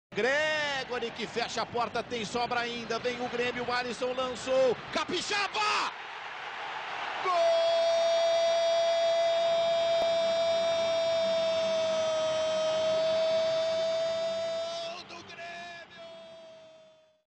Gol do Grêmio - Gol de Juninho Capixaba - Grêmio 1 x 2 Bahia
Gol Grêmio
Gol de Juninho Capixaba
Campeonato Brasileiro Grêmio x Bahia